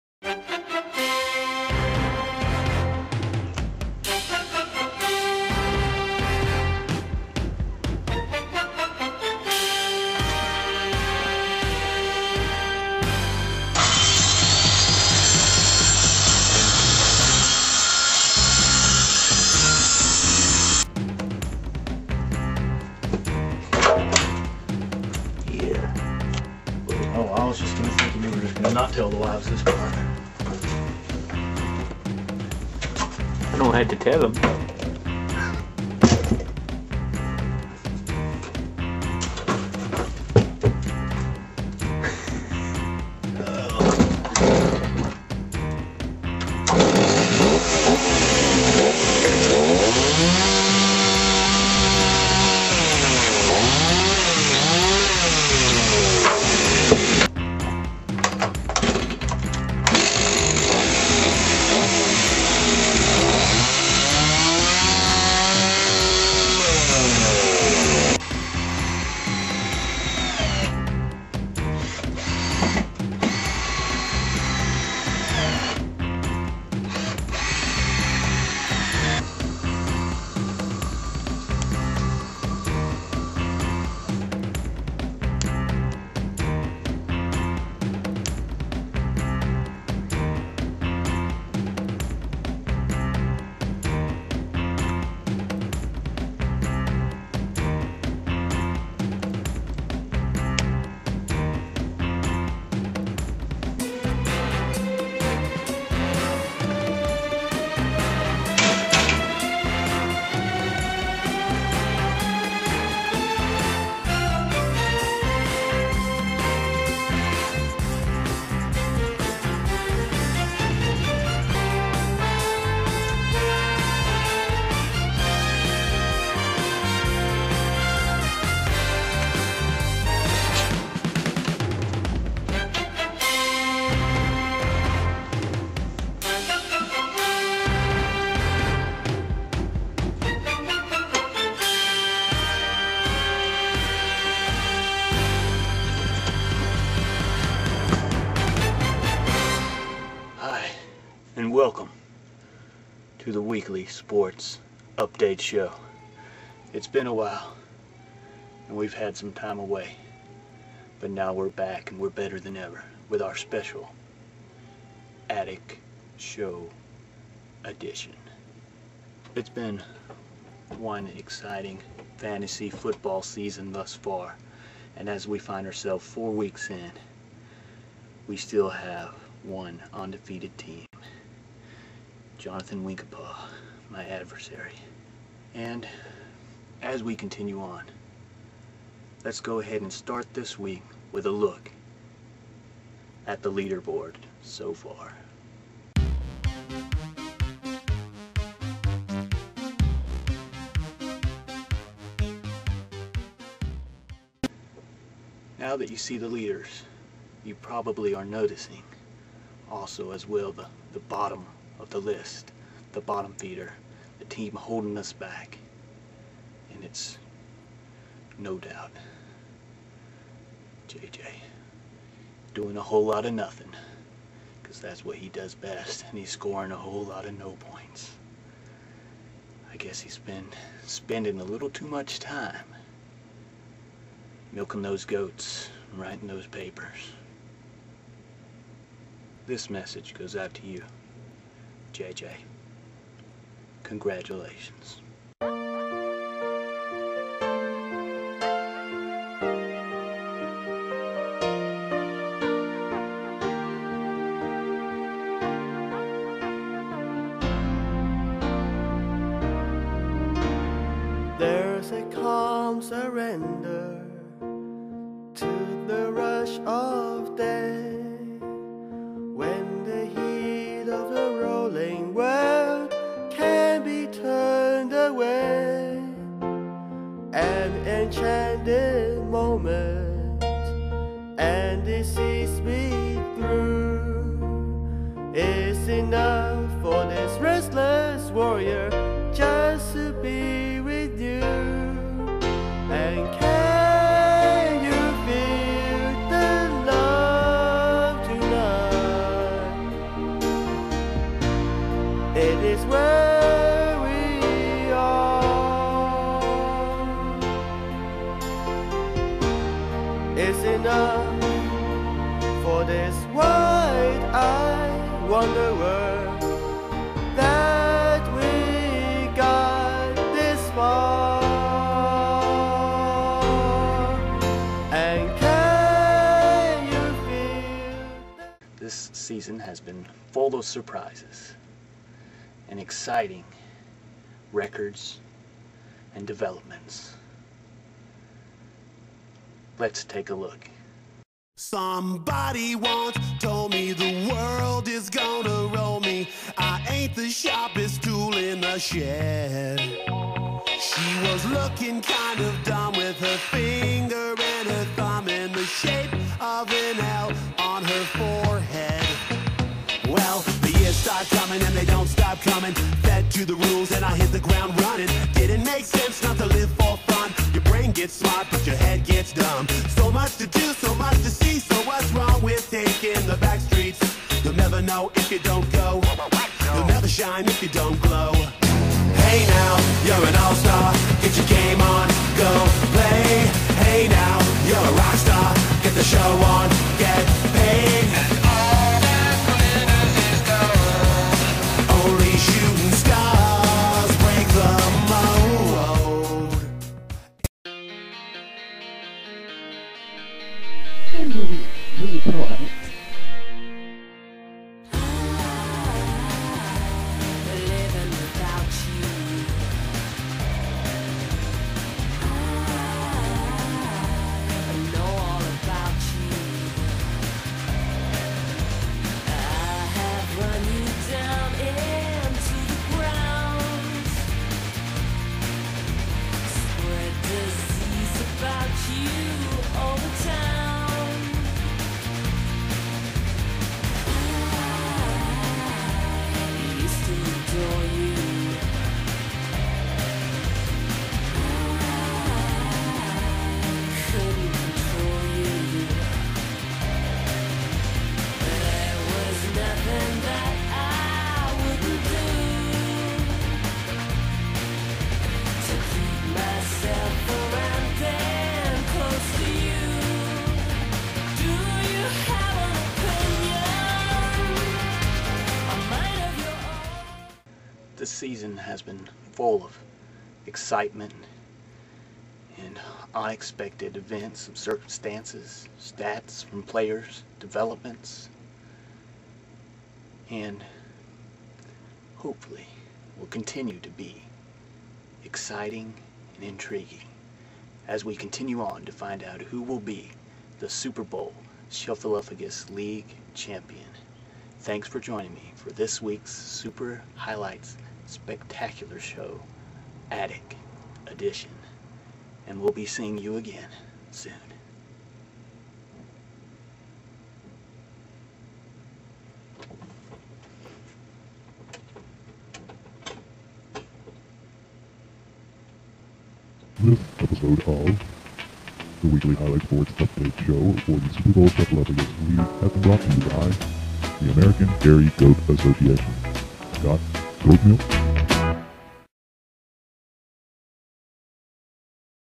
4 Weeks Scoreboard Show
Updating the Super Bowl Shuffleupagus Fantasy Football League with highlights, music, photos, video, and commentary on the season standings as they stand after 4 competitive weeks.